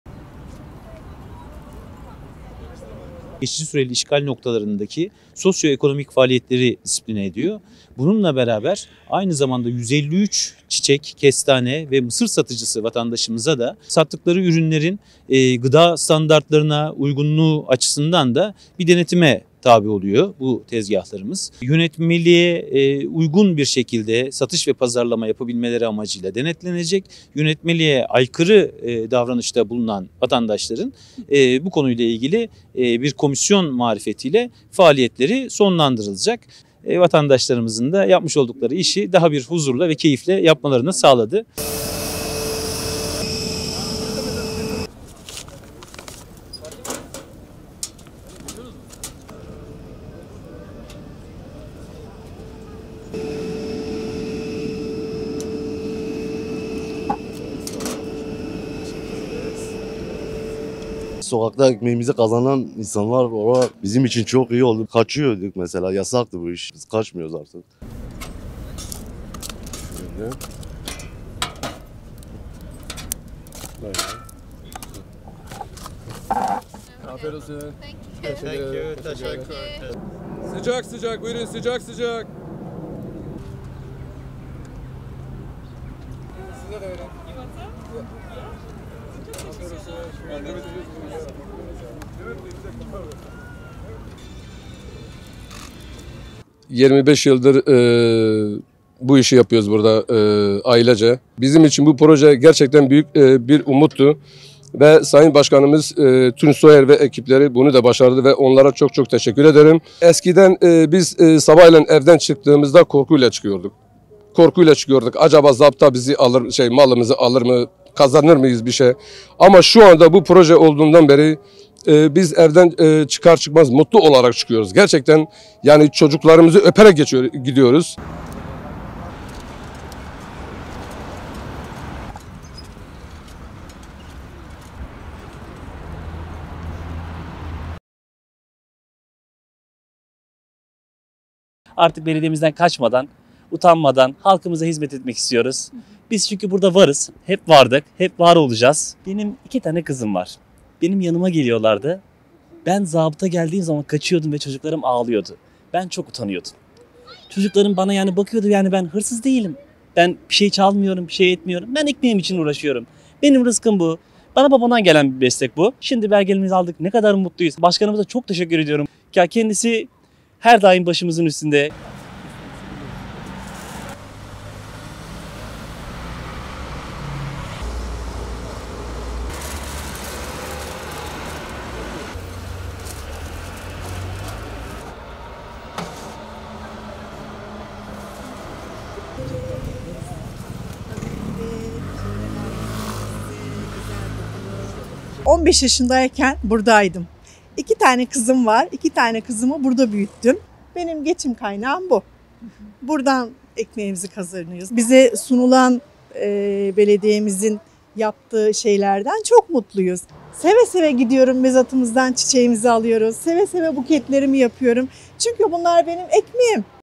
İzmir'de Sokak Emekçileri Huzura Erdi
Birer kooperatif çatısı altında birleşen kestane, mısır ve çiçek satıcıları, İzmir Büyükşehir Belediyesi Meslek Fabrikası’ndaki hijyen ve pazarlama eğitiminin ardından belirlenen noktalarda satışa başladı.